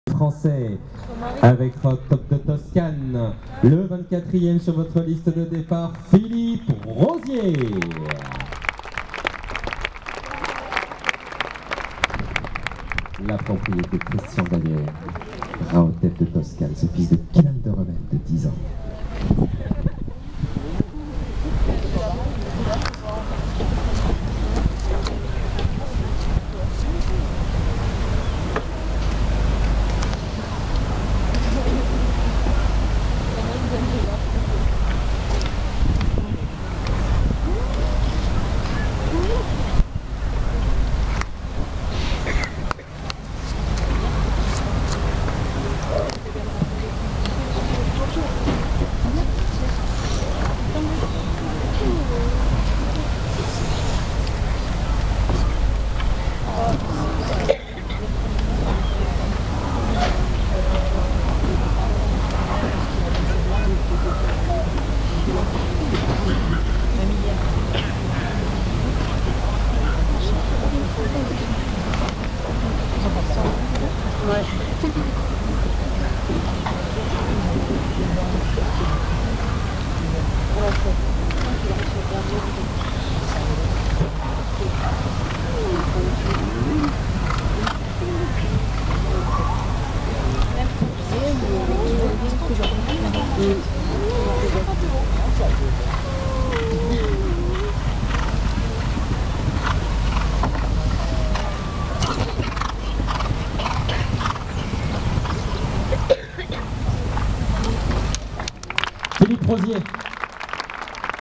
Philippe Rozier et Rahotep de Toscane- Chantilly 2015 CSI 5*
4 points lors du GP dominical 150 cm pour Philippe Rozier et Rahotep de Toscane- Chantilly 2015